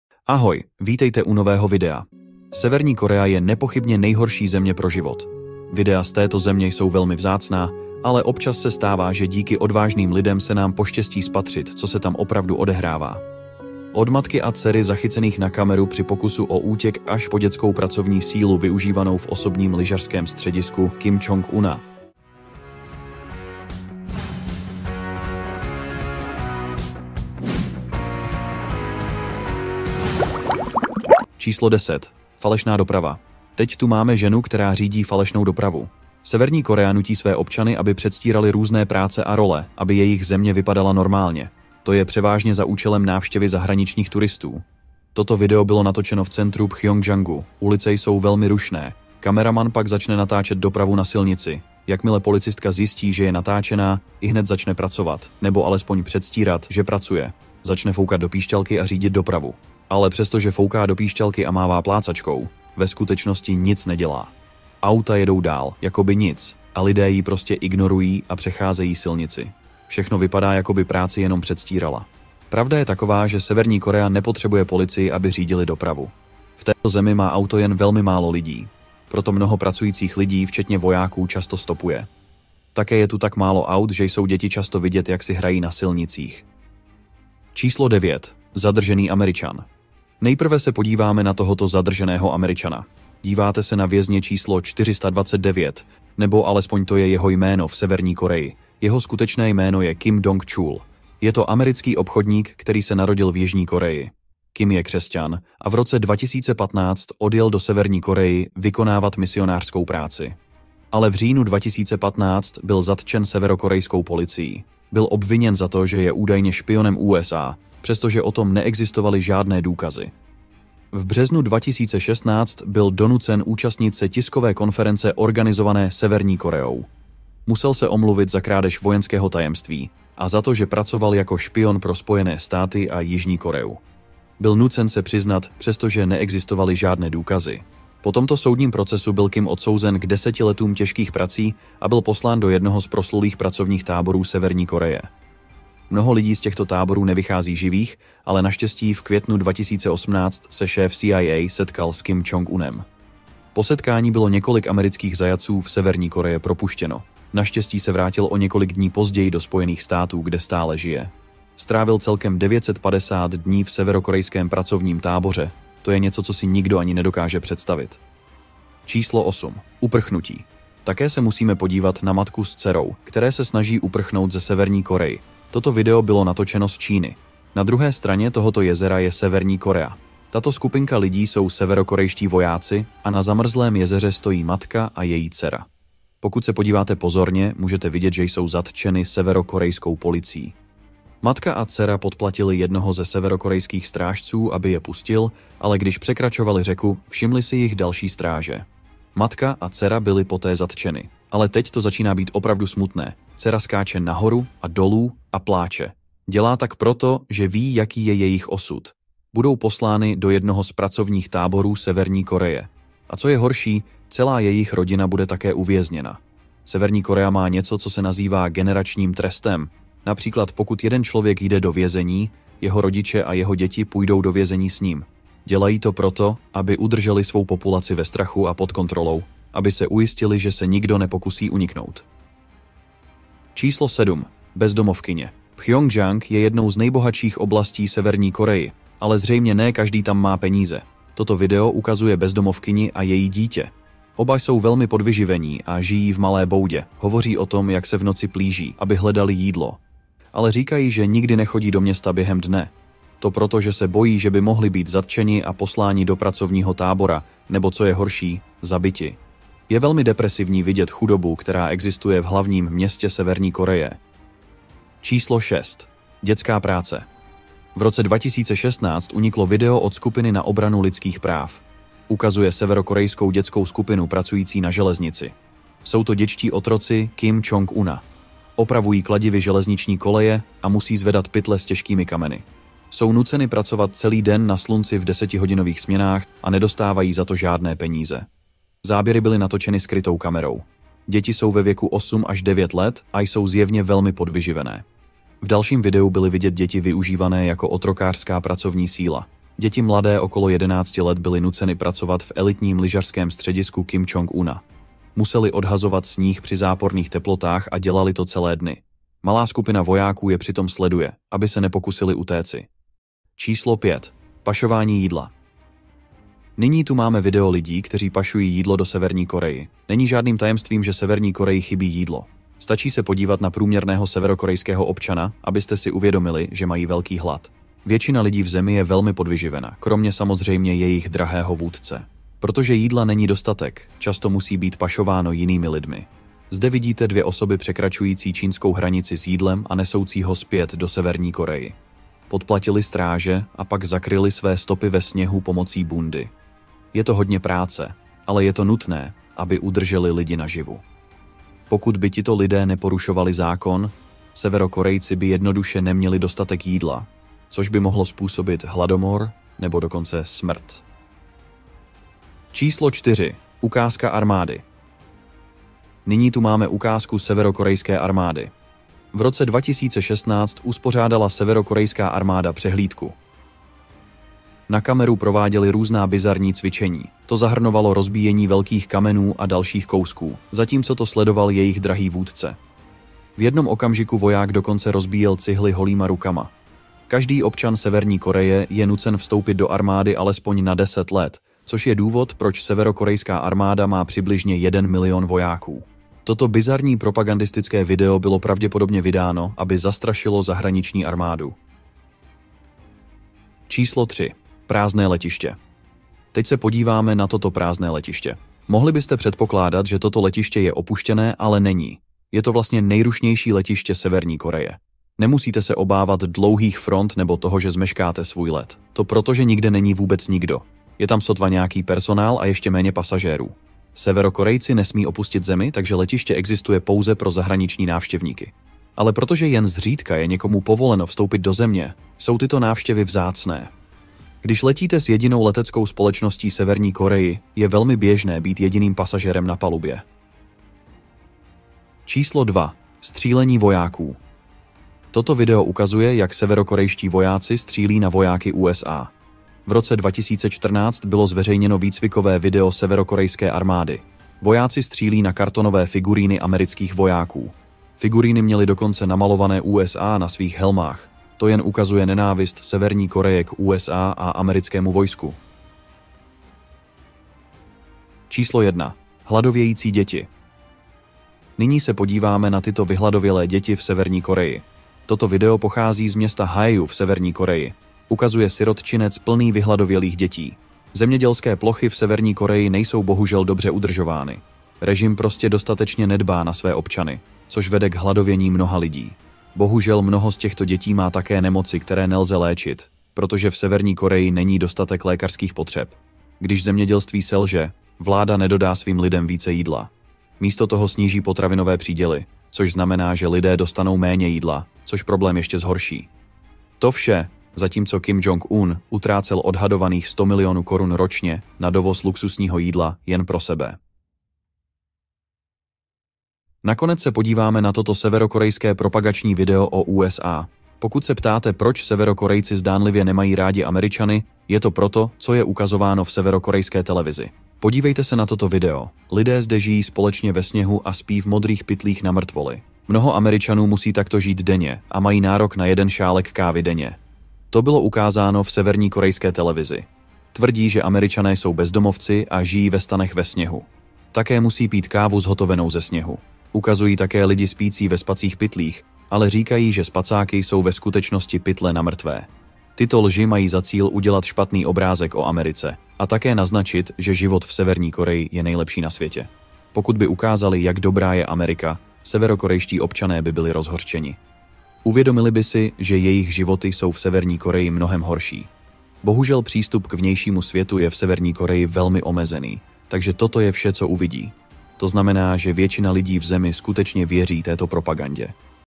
10 Videí Které Chce Severní Korea Smazat z Internetu
Korejská lidově demokratická republika, známější jako Severní Korea, je státem zahaleným v tajemství a izolovaným od okolního světa. Přesto několik odvážných jedinců riskuje svůj život, aby uprchli přes hranice a unikli tomuto uzavřenému režimu. V dnešním videu si představíme 10 šokujících záznamů, které Severní Korea chce smazat z Internetu, včetně příběhů 5 přeběhlíků, kterým se útěk povedl.